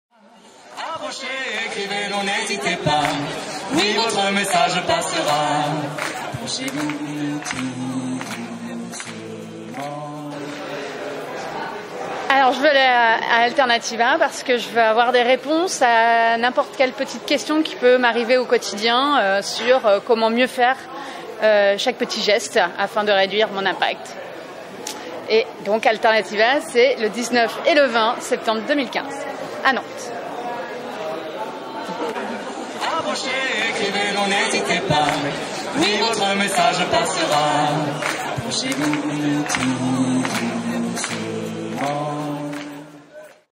Un week-end avec Stéphanie?
Stéphanie sera à Nantes les 19 et 20 septembre 2015!

Oui au respect de l'avenir, en finir avec les accumulations. Oui à la vie, oui aux sourires, en finir avec les compétitions. Oui à un environnement vivable.

BIEN VIVRE, ENFIN!